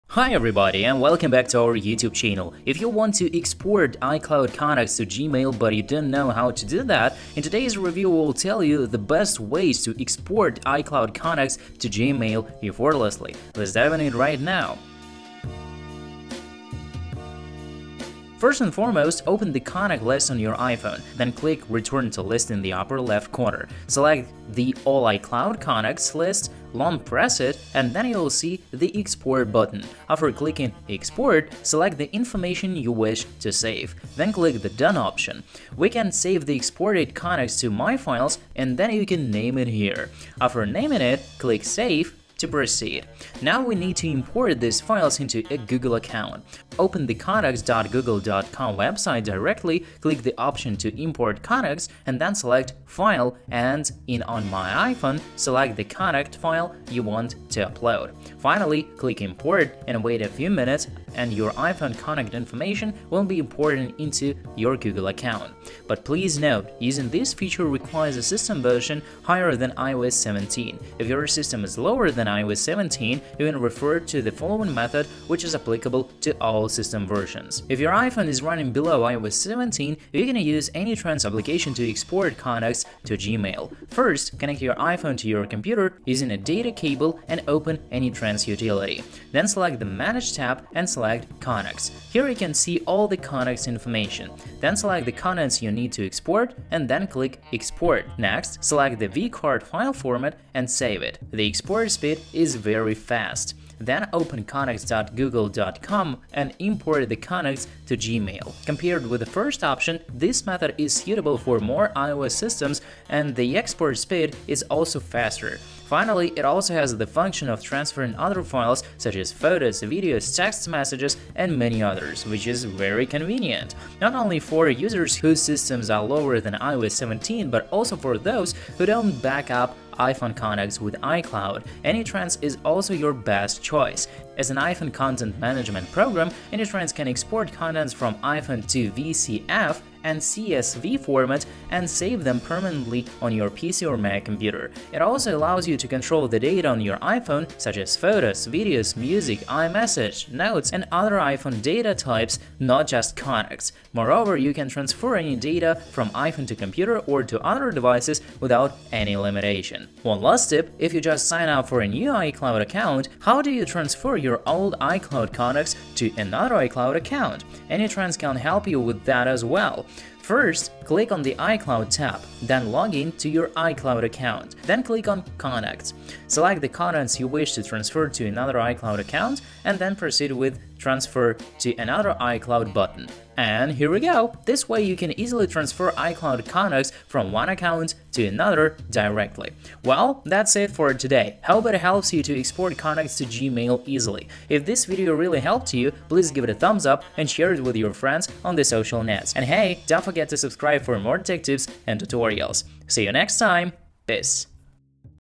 How to Export iPhone Contacts to Gmail | Import iPhone Contacts in Google Account
In today's video, we will tell you the best way to export iCloud contacts to Gmail.

If you want to transfer contacts to Gmail quickly, watch this video. We will tell you how to transfer using a computer or directly using your phone, as well as the extra tips at the end of the video that will definitely help you.
00:00 Intro
00:19 Export Contact Directly (iOS17 and Above)
01:19 Use AnyTrans to Export Contacts to Gmail (iOS 15/16/17/18 Compatible)
02:40 Bonus Tip: How to Transfer iCloud Contacts to Another iCloud Account